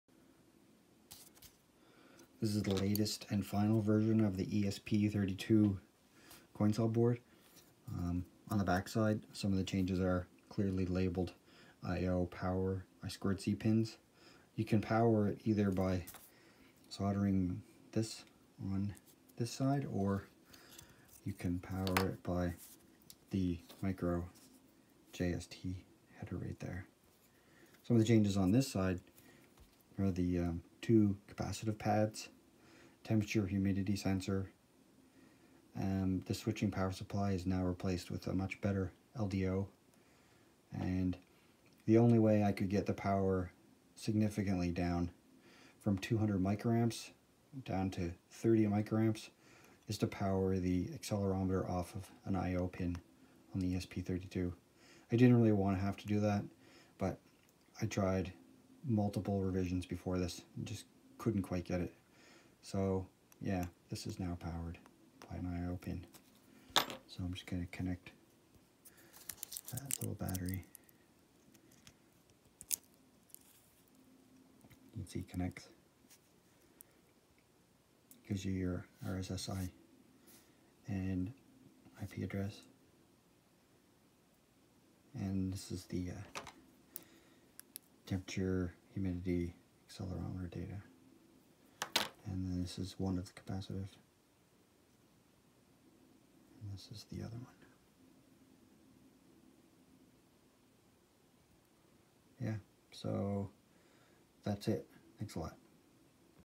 ESP32 CoinCell Final Revision
Version 4 of the ESP32 CoinCell is my final version of this project. I've decided to move on with my life. Trying to get the various parts all drawing as less power as possible has been a difficult but great learning experience. Previous versions using switching power supplies, LDOs and accelerometers drawing way too much quiescent current as come to this. Revision 4 is offers these features:
- two capacitive touch pads on the topside
- clearly labeled user accessible  GPIO, power and i2c pads on the bottom side
- the ability to power it with a coincell or external battery
- a temperature/humidity sensor
- the accelerometer powered off of an ESP32 GPIO pin
- 30uA sleep current